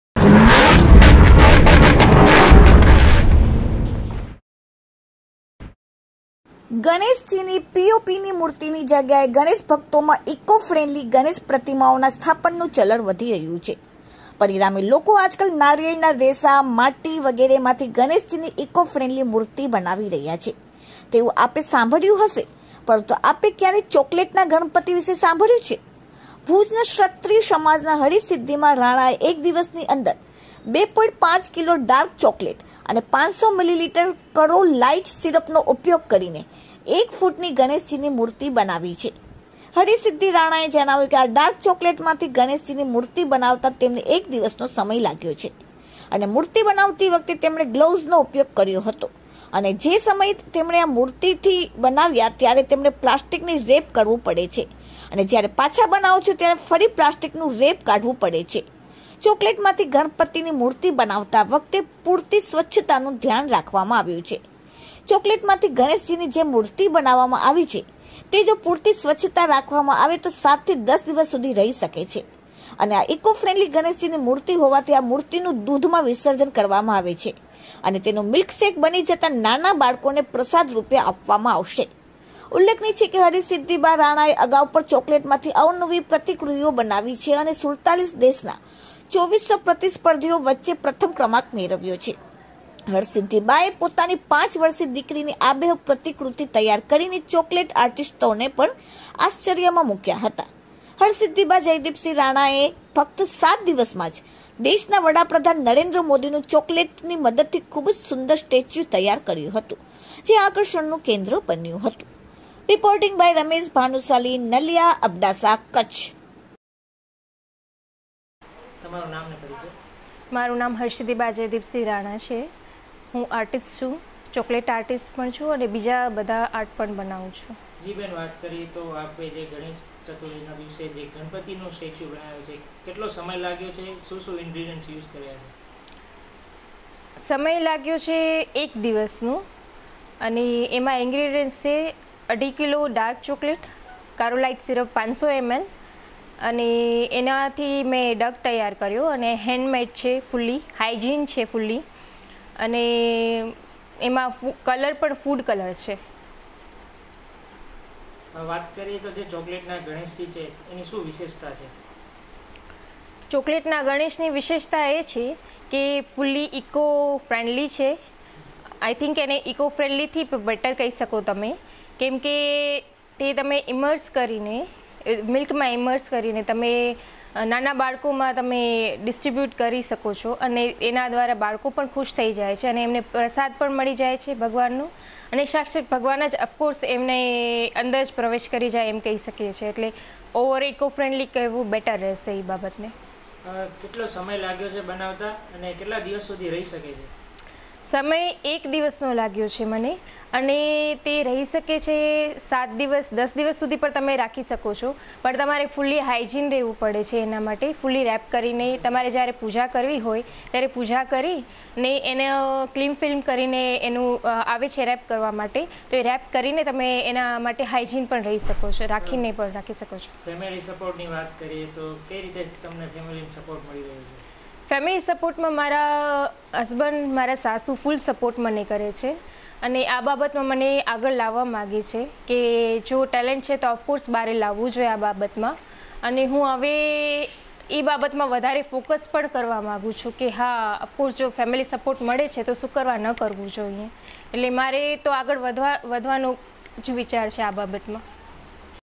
KUTCH ગણેશજીની પીઓપીની મૂર્તિની જગ્યાએ ગણેશભક્તોમાં ઇકો ફ્રેન્ડલી ગણેશ પ્રતિમાઓના સ્થાપનનું ચલણ વધી
ગણેશજીની પીઓપીની મૂર્તિની જગ્યાએ ગણેશભક્તોમાં ઇકો ફ્રેન્ડલી ગણેશ પ્રતિમાઓના સ્થાપનનું ચલણ વધી રહ્યું છે.

 પરિણામે લોકો આજકાલ નારિયેળના રેસા, માટી વગેરેમાંથી ગણેશજીની ઇકો ફ્રેન્ડલી મૂર્તિ બનાવી રહ્યા છે તેવું આપે સાંભળ્યું હશે પરંતુ આપે ક્યારેય ચોકલેટના ગણપતિ વિશે સાંભળ્યું છે.ભુજના ક્ષત્રિય સમાજના હરસિધ્ધિબા રાણાએ 1 દિવસની અંદર 2.5 કિલો ડાર્ક ચોકલેટ અને 500 ml karo light syrup નો ઉપયોગ કરીને 1 ફૂટની ગણેશજીની મૂર્તિ બનાવવામાં આવી છે.

હરસિધ્ધિબા રાણાએ જણાવ્યું હતું કે, આ ડાર્ક ચોકલેટમાંથી ગણેશજીની મૂર્તિ બનાવતા તેમને 1 દિવસનો સમય લાગ્યો છે અને મૂર્તિ બનાવતી વખતે તેમને ગ્લવઝ નો ઉપયોગ કર્યો હતો અને જે સમયે તમે આ મૂર્તિ નથી બનાવતા ત્યારે તેને પ્લાસ્ટિકથી wrap કરવું પડે છે અને જ્યારે પાછા બનાવો છો ત્યારે ફરી પ્લાસ્ટિકનું wrap કાઢવું પડે છે.ચોકલેટમાંથી ગણપતિની મૂર્તિ બનાવવા વખતે પૂરતી સ્વચ્છતાનું ધ્યાન રાખવામાં આવે છે.

ચોકલેટમાંથી ગણેશજીની જે મૂર્તિ બનાવવામાં આવી છે તે જો પૂરતી સ્વચ્છતા રાખવામાં આવે તો 7 થી 10 દિવસ સુધી રહી શકે છે.અને આ ઇકો ફ્રેન્ડલી ગણેશજીની મૂર્તિ હોવાથી આ મૂર્તિનું દૂધમાં વિસર્જન કરવામાં આવે છે અને તેનું મિલ્કશેક બની જતા નાના બાળકોને પ્રસાદ રૂપે આપવામાં આવશે.

ઉલ્લેખનીય છે કે હરસિધ્ધિબા રાણાએ અગાઉ પણ ચોકલેટમાંથી અવનવી પ્રતિકૃતિઓ બનાવી છે અને 47 દેશના 2400 પ્રતિસ્પર્ધીઓ વચ્ચે પ્રથમ ક્રમાંક મેળવ્યો છે. હરસીધ્ધીબાએ પોતાની 5 વર્ષિય દીકરીની આબેહુબ પ્રતિકૃતિ તૈયાર કરીને ચોકલેટ આર્ટીસ્ટોને પણ આશ્ચર્યમા મુક્યા હતા.હરસિધ્ધિબા જયદીપસિંહ રાણાએ પછી ફક્ત 7 દીવસમા જ દેશના વડાપ્રધાન નરેંદ્ર મોદીનું ચોકોલેટની મદદથી ખુબજ સુંદર સ્ટેચ્યું તૈયાર કર્યુ હતું જે આકર્ષણનું કેન્દ્ર બન્યું હતું.

સ્ટોરી રમેશભાઈ ભાનુશાલી નલિયા અબડાસા કચ્છ